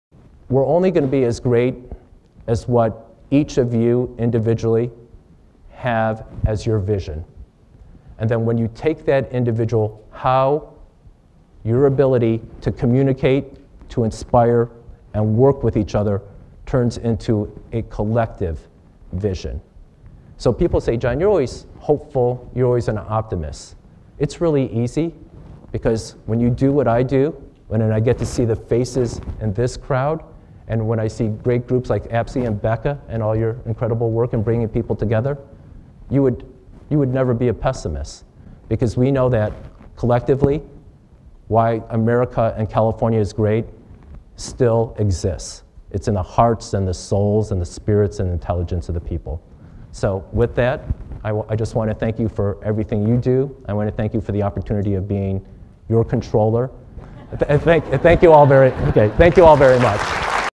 PEARLS OF INNOVATION: Controller John Chiang, State of California
PEARLS OF INNOVATION are short snippets that excite, inspire provoke the leading edge of thinking on innovative environments in government.  California State Controller John Chiang shared that each of will only be as great as our vision accompanied with our ability to come together to live and work and share our collective vision.  The reason why California and America is great lies in the heart, soul and spirit and intelligence of its people.